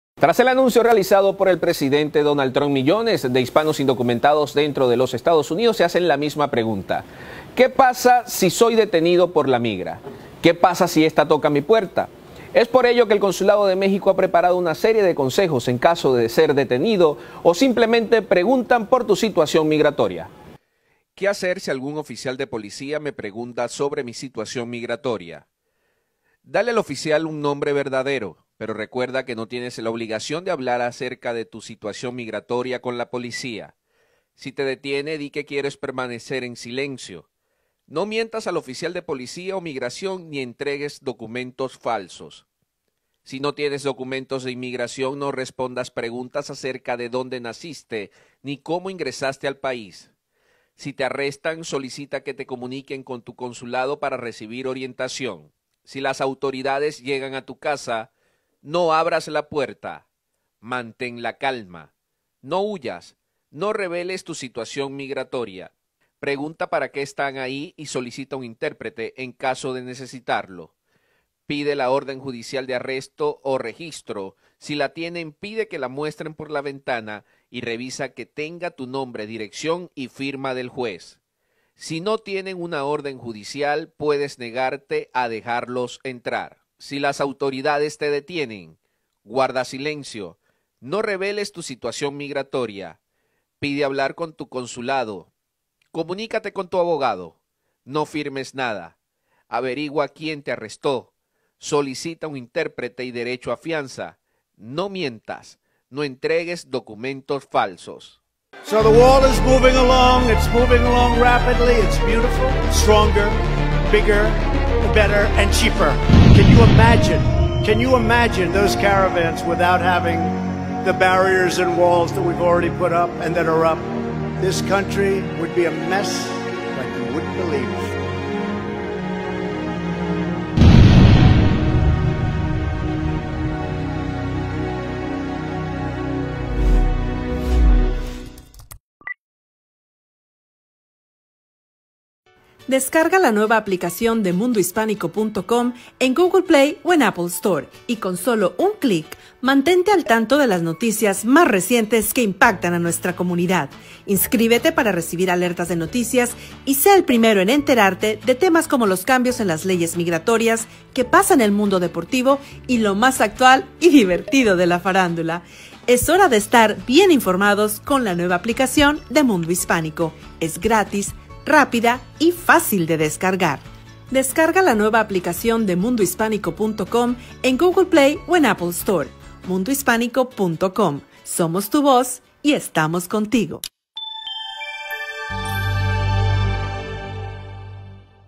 ¿Qué debe hacer si ICE llega a su hogar o su trabajo?
Después de los anuncios del presidente Trump sobre las masivas redadas que se realizaran para lograr deportar a millones de inmigrantes, la comunidad hispana vive en alerta, pues ya no se sienten seguros debido a su estatus migratorio.

¿Sabe qué hacer si ICE toca la puerta de su casa o llega a su lugar de trabajo?
¿Qué debe hacer si 'La Migra' lo detiene?

Le contamos que procedimientos debe seguir, los mejores consejos para cuando se encuentre cara a cara con un agente federal.